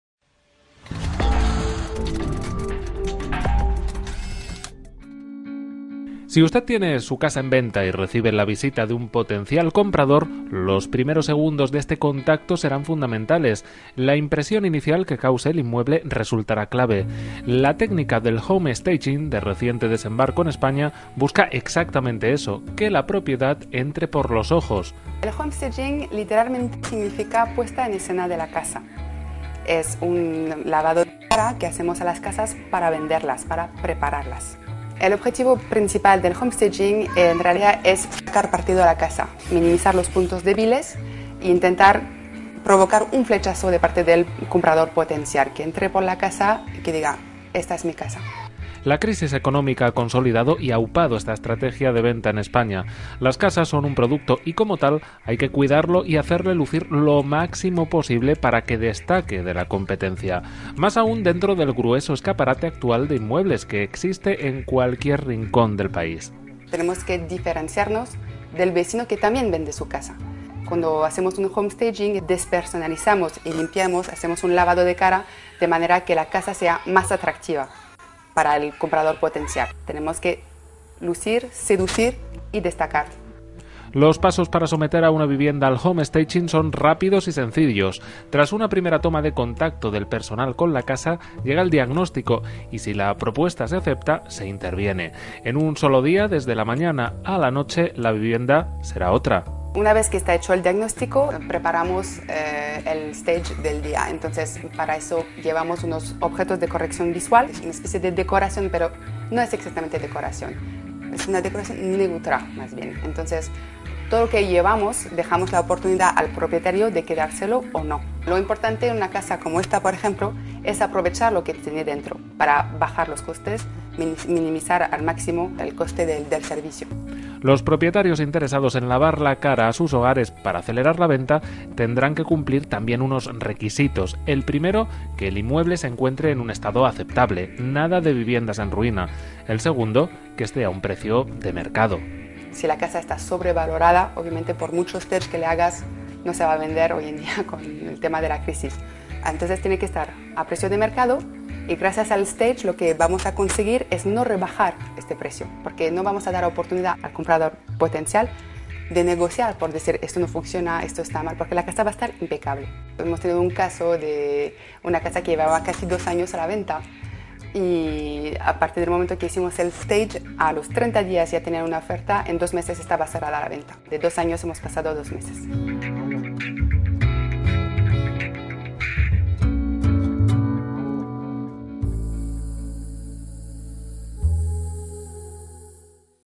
home staging | El flechazo como técnica de venta rápida de casas
La primera impresión también cuenta, y mucho, al comprar casa. Los inmuebles en venta además de seducir deben destacar. Tienen que causar un flechazo a primera vista. La técnica home staging, importada de Estados Unidos y centrada en mejorar la imagen de las viviendas, se hace hueco en el paralizado mercado español con el objetivo de conquistar, por los ojos, al potencial comprador.

Le esperamos en los teléfonos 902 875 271 ó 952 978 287 ó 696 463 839 (llamar a cualquier hora).